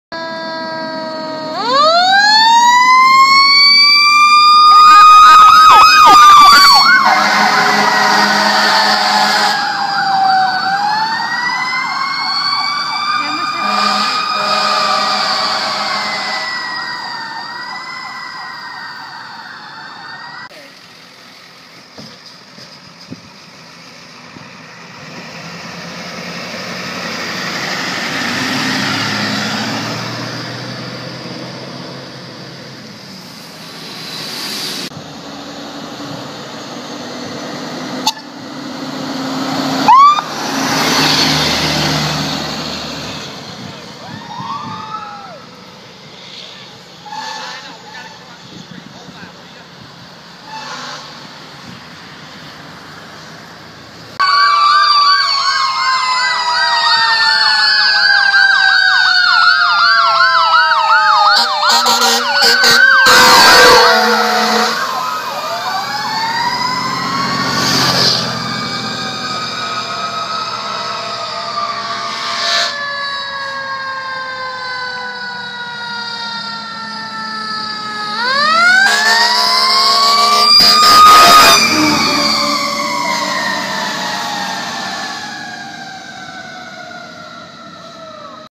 Firetruck Drive Bys
Firetrucks leaving parade with flashing lights and blaring sirens. Litchfield, CT September 21, 2014